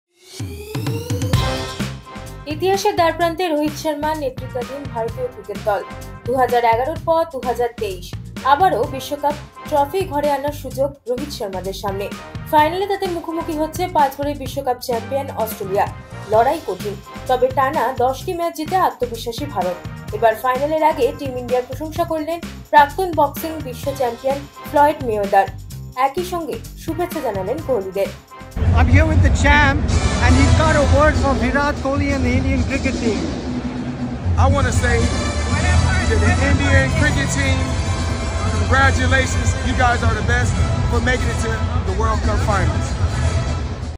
ICC World Cup 2023: ভারতকে শুভেচ্ছা ফ্লয়েড মেওয়েদারের, কী বার্তা দিলেন বক্সার কিংবদন্তী?
ইতিহাসের দ্বারপ্রান্তে রোহিত শর্মার নেতৃত্বাধীন ভারতীয় ক্রিকেট দল । ২০১১-এর পর ২০২৩ । আবারও বিশ্বকাপ ট্রফি ঘরে আনার সুযোগ রোহিত শর্মাদের সামনে । ফাইনালে তাঁদের মুখোমুখি হচ্ছে পাঁচবারের বিশ্বকাপ চ্যাম্পিয়ন অস্ট্রেলিয়া । লড়াই কঠিন ।